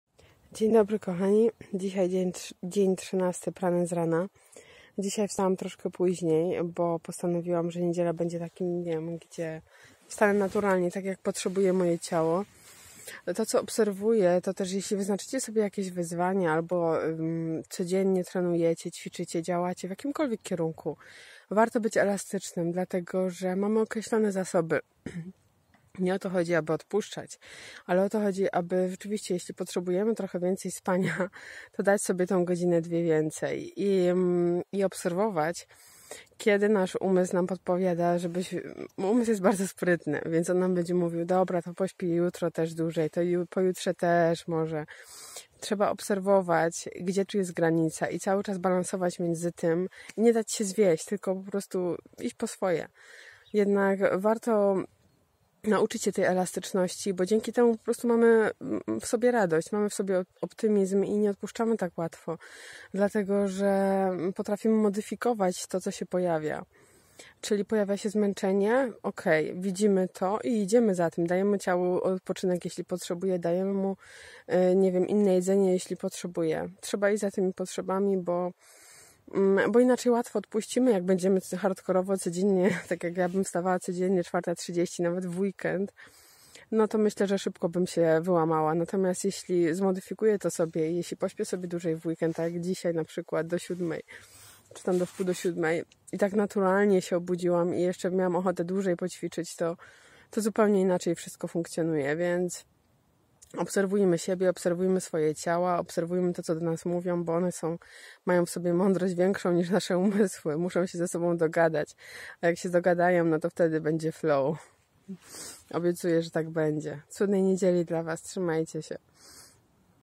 Dz.13 PRANAZRANA Elastyczność
Dz. 13
Elastyczność
❤️Dzisiaj niedziela więc moje święto. Niedziela jest dla mnie dniem kiedy śpię tyle ile chcę,  dzisiaj do 6.30 i biegam dłużej👊👊🏋️🍀

Pamiętaj, że cokolwiek sobie wyznaczysz jako cel w swoim życiu, elastyczność będzie Ci potrzebna aby utrzymać się na fali.
Dlaczego???
Uwaga! To nie odpuszczanie 😊😊😊
Chodzi o to, aby sam proces był dla nas radością❤️❤️🍒🥑 niby takie banalne , ale ja nie potrafię z innego poziomu działać.
Kiedy widzę, że leniuszek się pojawia ruszam do działania. Jednak kiedy widzę, że moje ciało potrzebuje naprawdę odpocząć daje mu to. Przecież moje Ciało tak pięknie mi służy ❤️❤️❤️
Więc zapamiętaj, że sam proces doskonalenia siebie jest radością. I jeśli to naprawdę czujesz, to zaufaj mi cokolwiek sobie postanowisz, tak będzie. A jak nie będzie 😊 to będzie inaczej, ale na pewno nie gorzej.
Otwórz się na różne sposoby i dbaj o radość w sercu. To dopiero motywacja 👊😘💜🙏